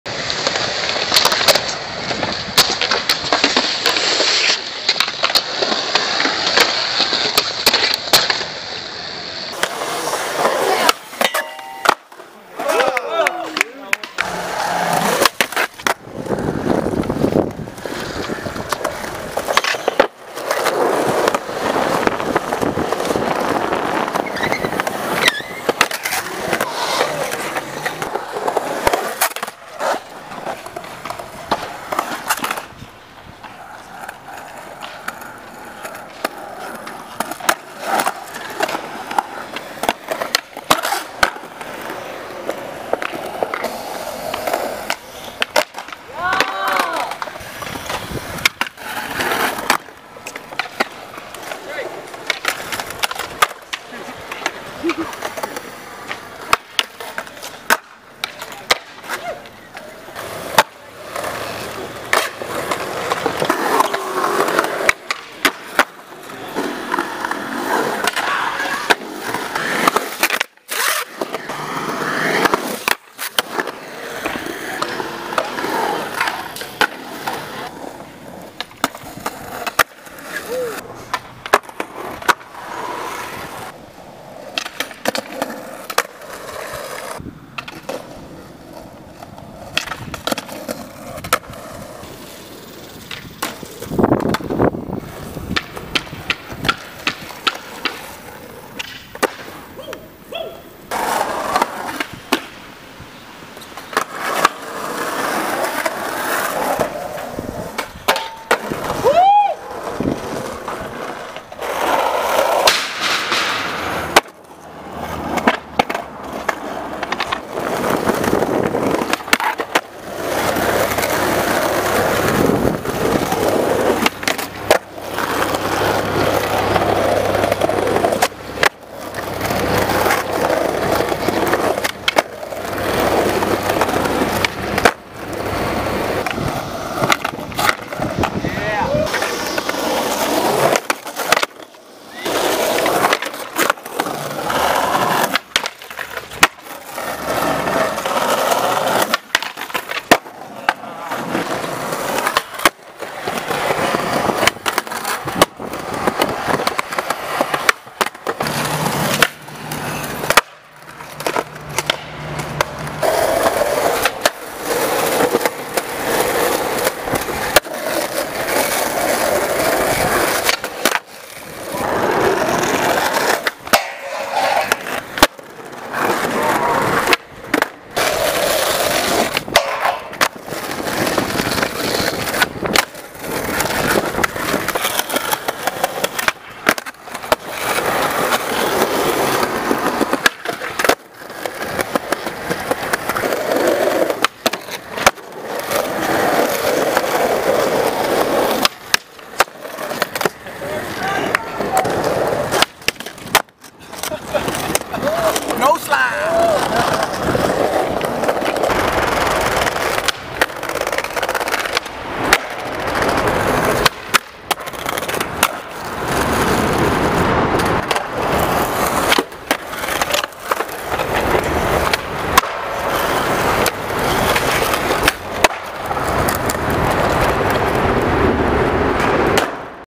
free
Some old,  some new and some random things over the last year. feat: Josh Pickle, Dee, Anthony Green, Nic Lott, Johnathan McKenzie, Henry Diaz, Josh Johnson, Jason Clancy, David Duvall & Chris Mattes.